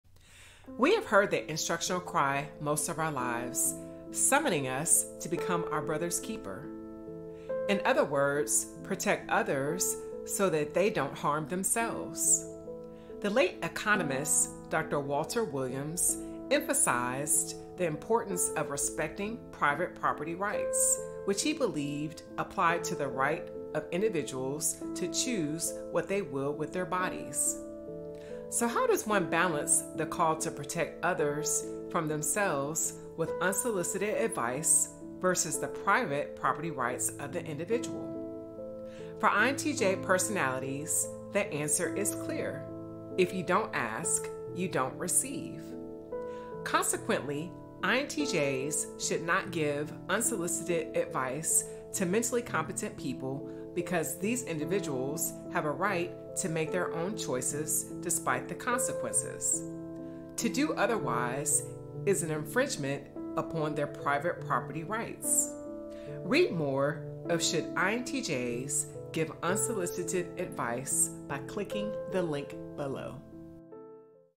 Should INTJs Give Unsolicited Advice?
The late economist Dr. Walter Williams emphasized the importance of respecting private property rights, which he believed applied to the right of individuals to choose what they will with their bodies.

So, how does one balance the call to protect others from themselves with unsolicited advice versus the private property rights of the individual?